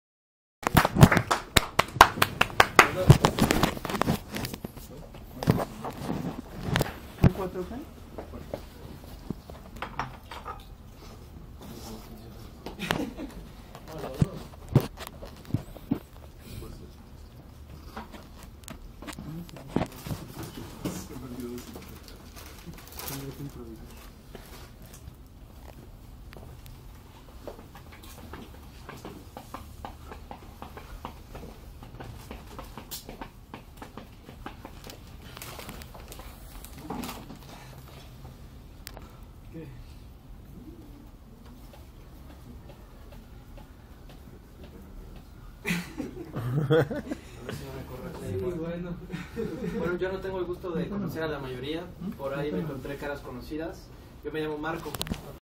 Meetup Karaokulta DFº
Primer MeetUp de KaraOKulta DF, llevado a cabo el 7 de Junio del 2012.
Inter entyre la primera presentación y la segunda presentación